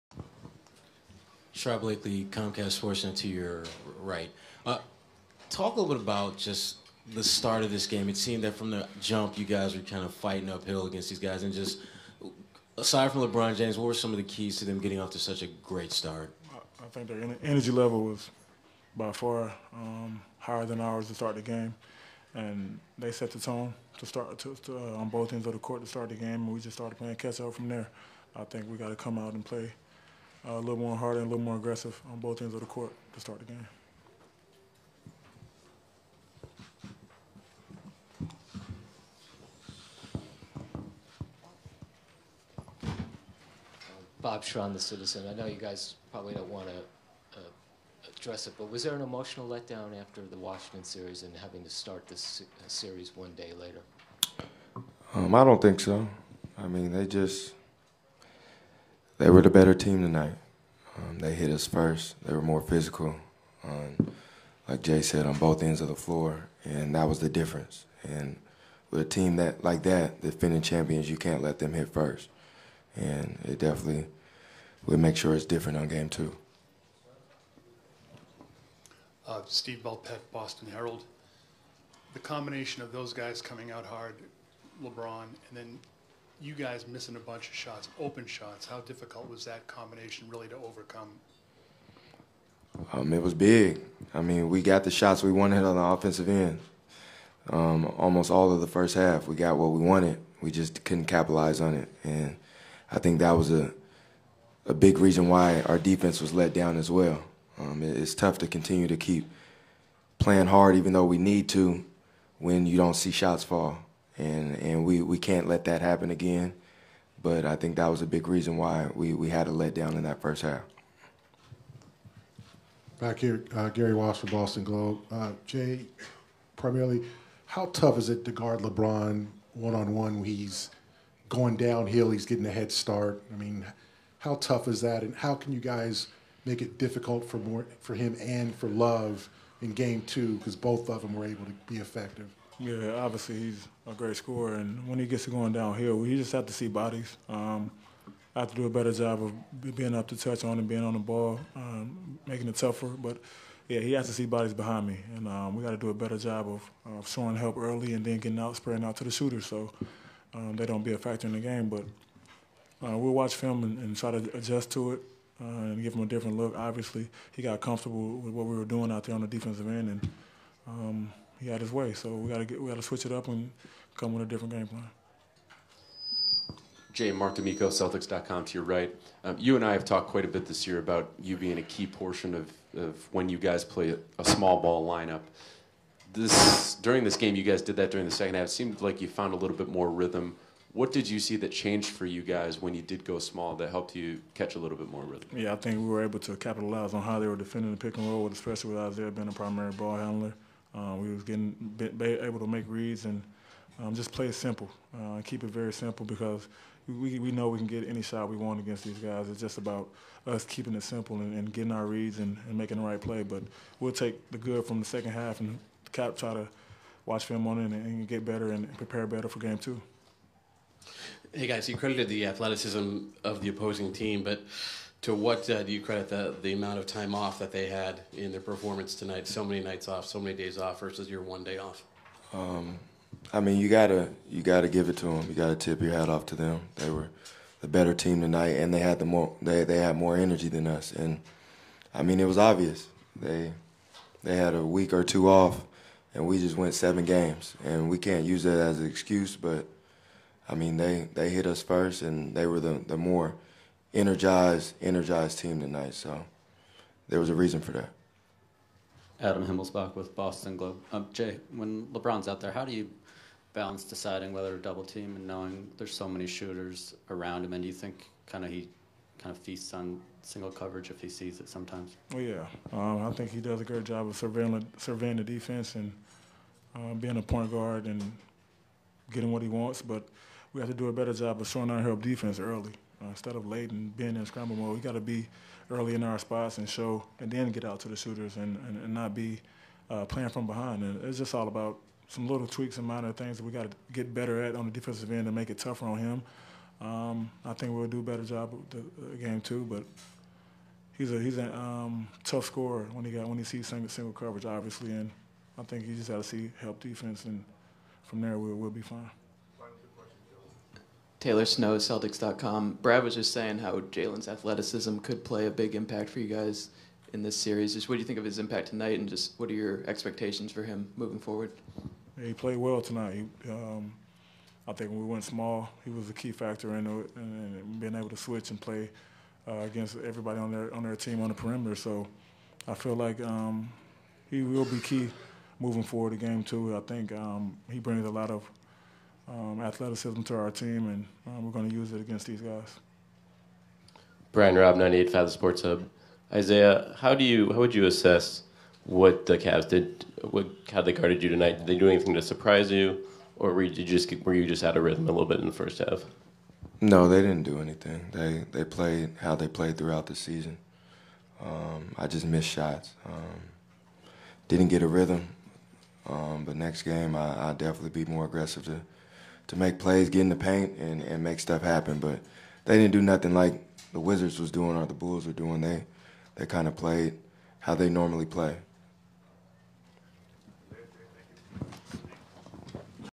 Jae Crowder & Isaiah Thomas On Slow Start, CAN'T GUARD LEBRON & Says Rest Played A Big Part
And don't forget to drop a like on the video.